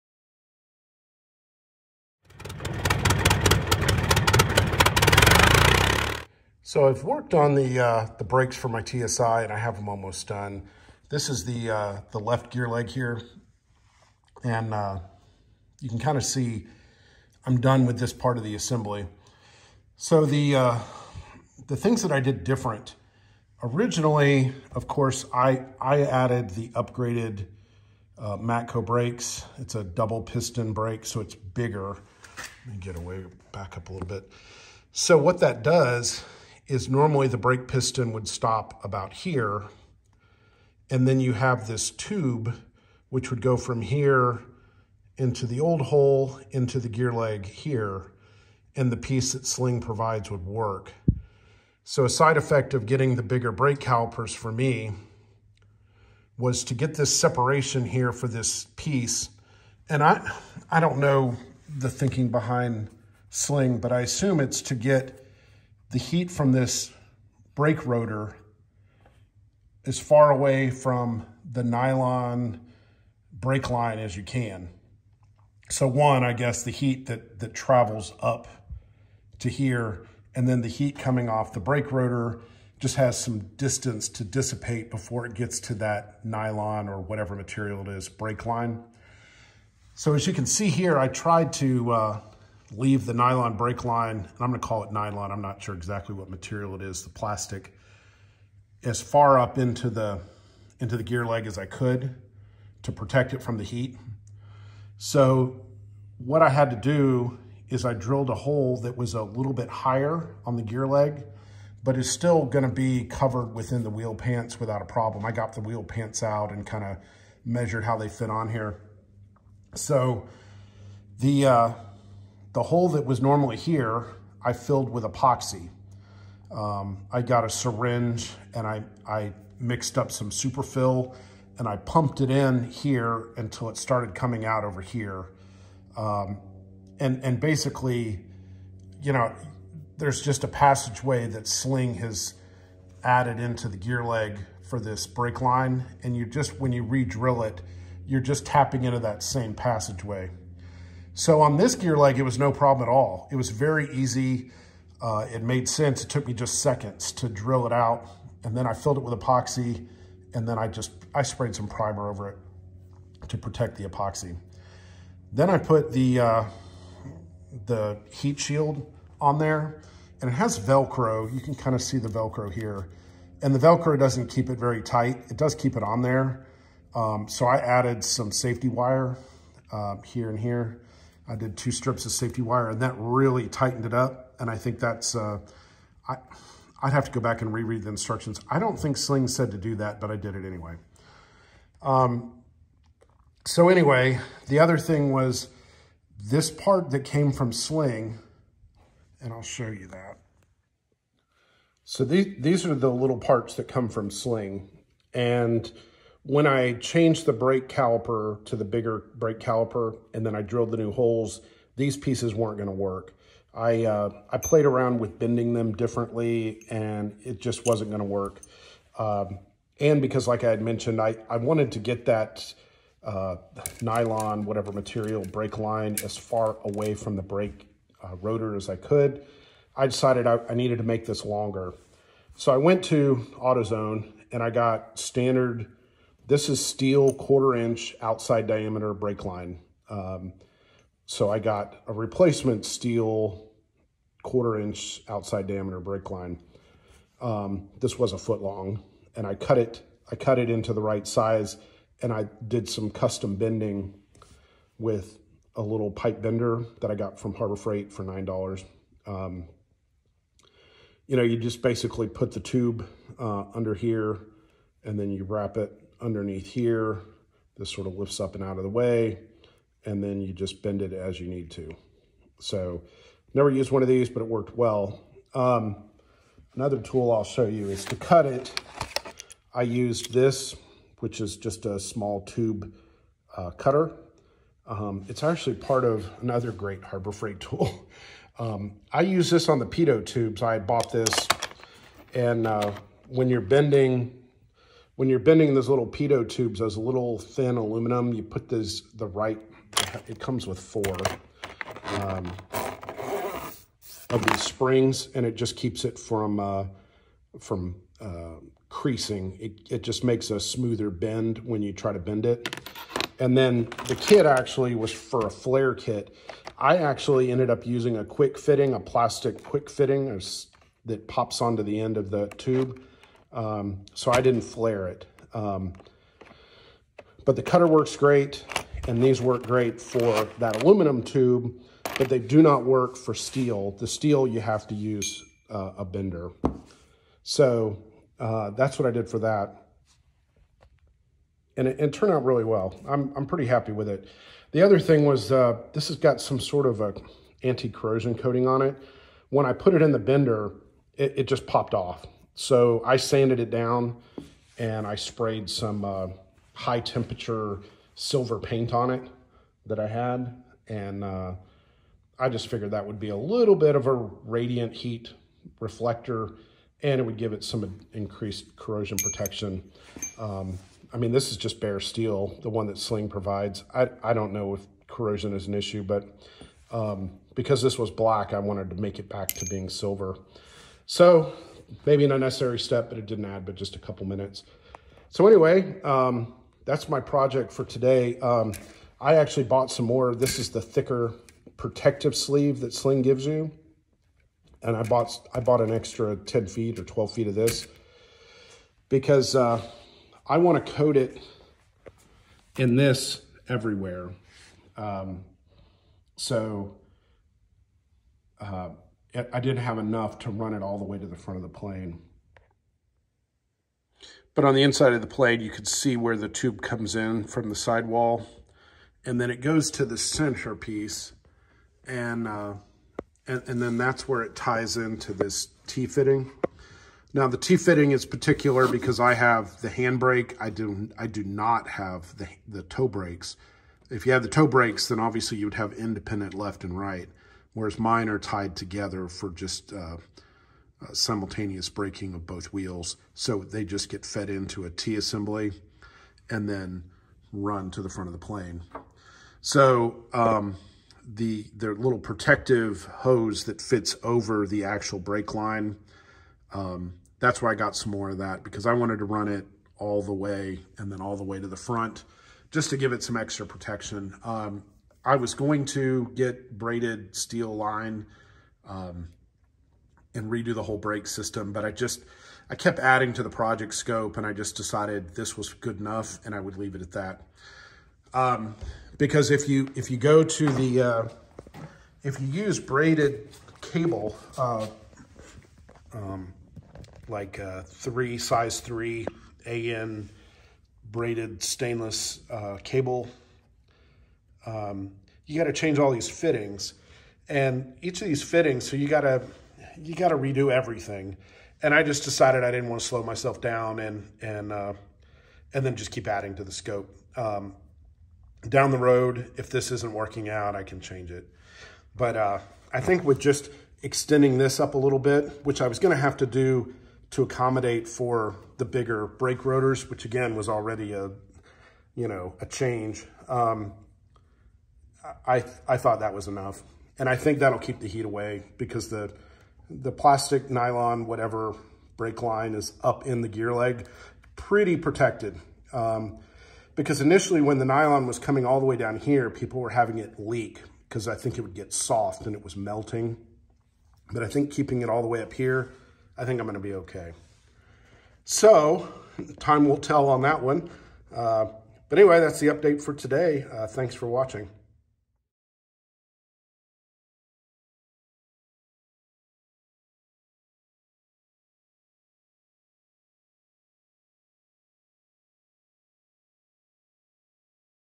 Sling TSI Build Update - Brakes
Sling TSI Build Update, Brake installation.  Explain changes required when using larger brakes on the Sling TSI.  Kit Built Experimental Aircraft Construction. Harbor Freight Tube Bender and Harbor Freight Tube Flare and bending kit.